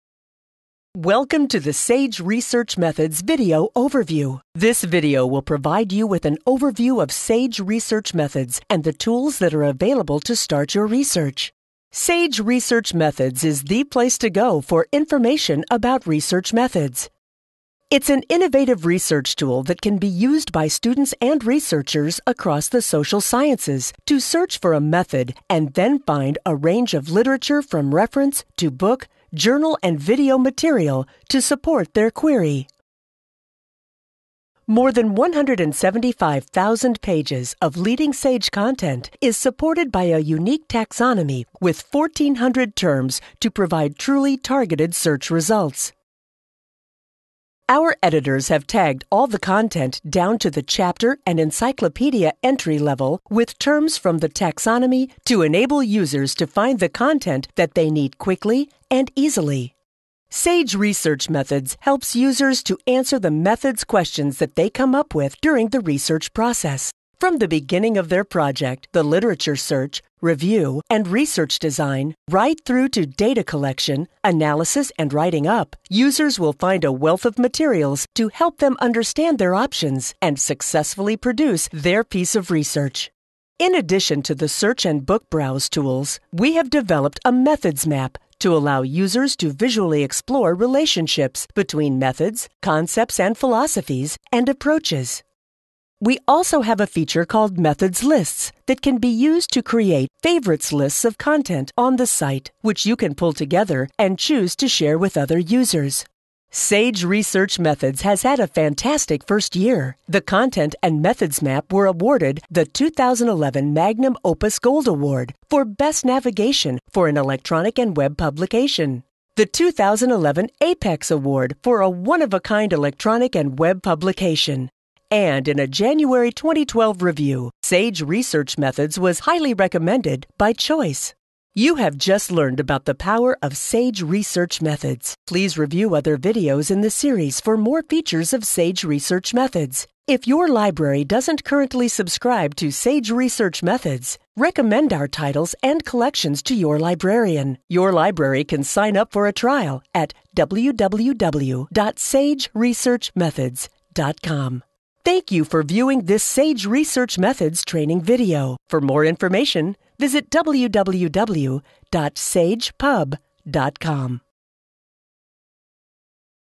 SAGE Research Methods Overview
SAGE Research Methods is a research methods tool created to help researchers, faculty and students with their research projects. SAGE Research Methods links over 175,000 pages of SAGE’s renowned book, journal and reference content with truly advanced search and discovery tools. Researchers can explore methods concepts to help them design research projects, understand particular methods or identify a new method, conduct their research, and write up their findings. Since SAGE Research Methods focuses on methodology rather than disciplines, it can be used across the social sciences, health sciences, and more.